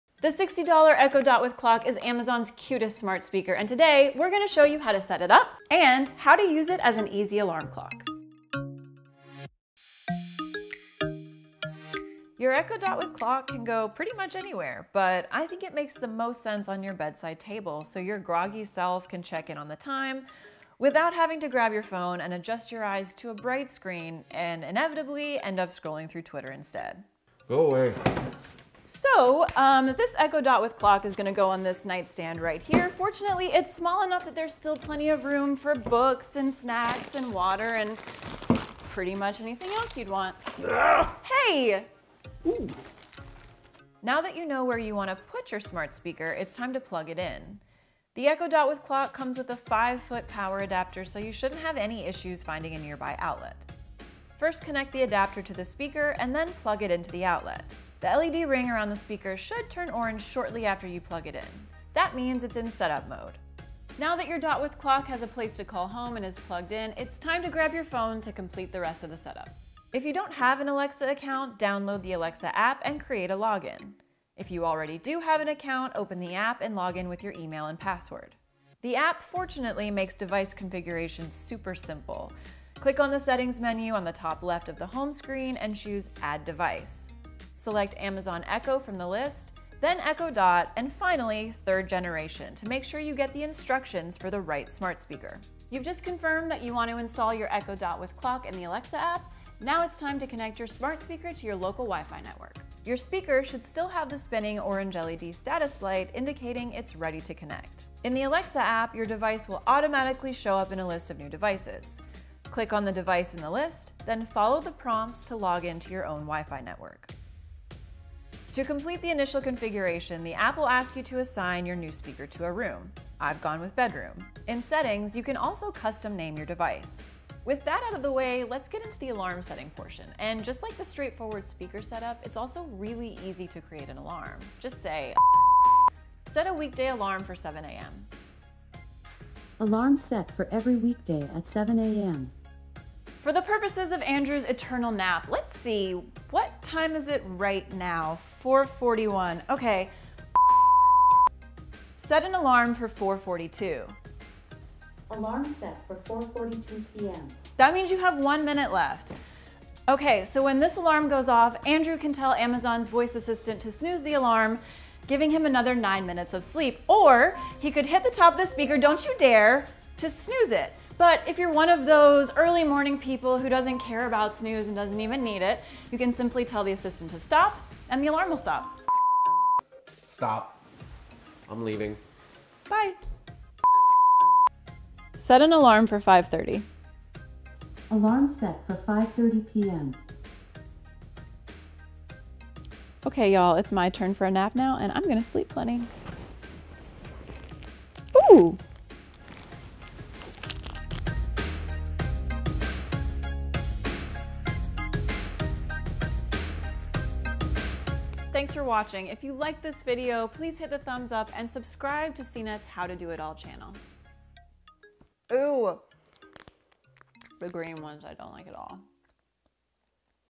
How to set up the Amazon Echo Dot with Clock
Learn how to set up your Echo Dot with Clock and create a custom wake-up alarm.

Megan’s twitter: @meganwollerton

CNET may get a share of revenue from the sale of the products linked above.